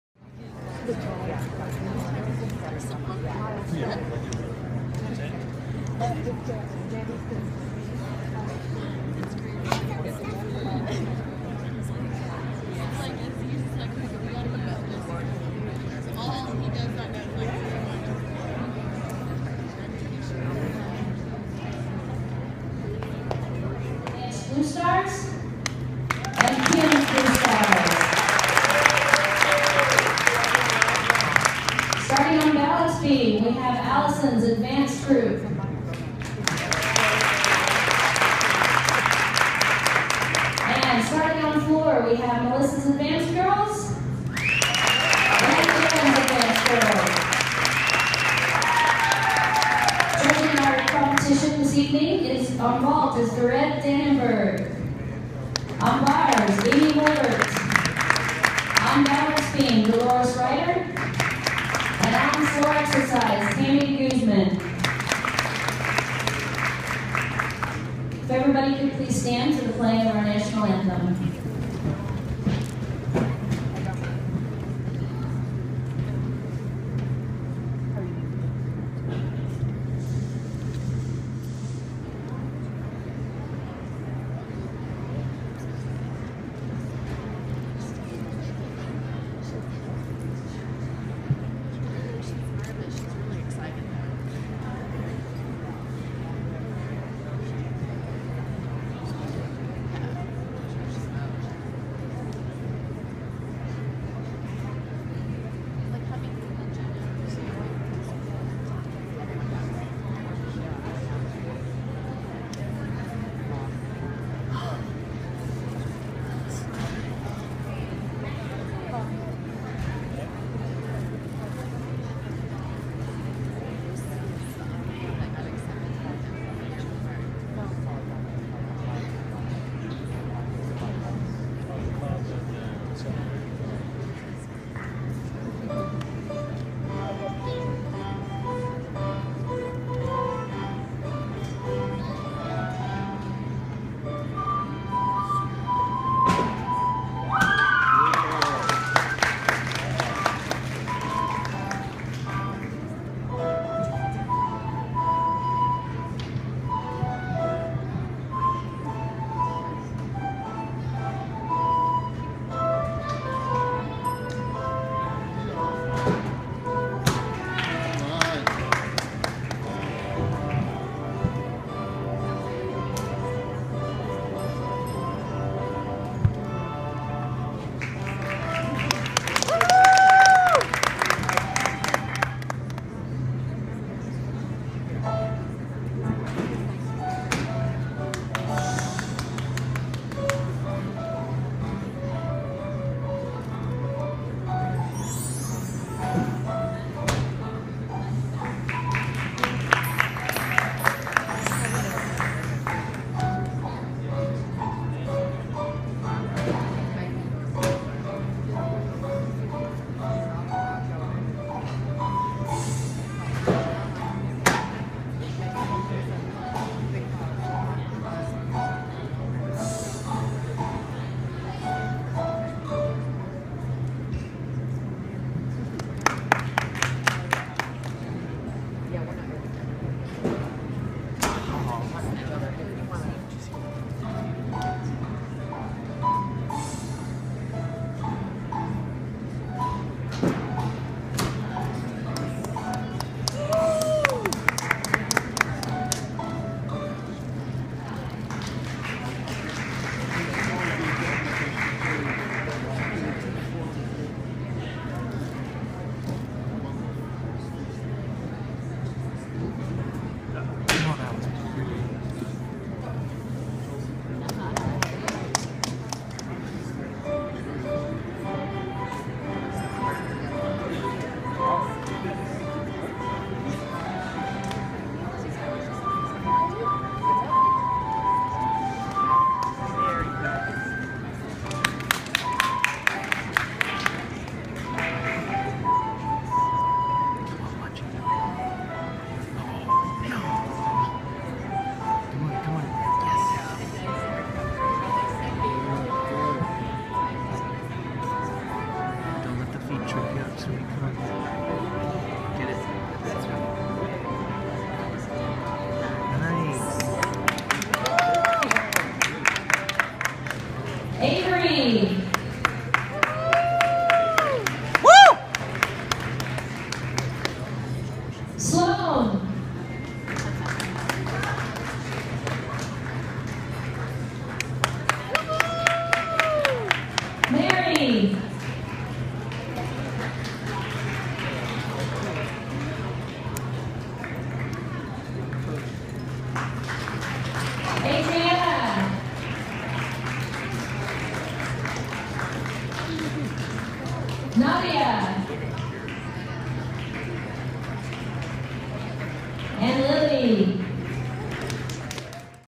Averi's first gymnastics meet
My daughter's first in house gymnastics meet. She has only been doing gymnastics for just shy of 1 year and I couldn't be prouder of her progress. Way to go sweet heart.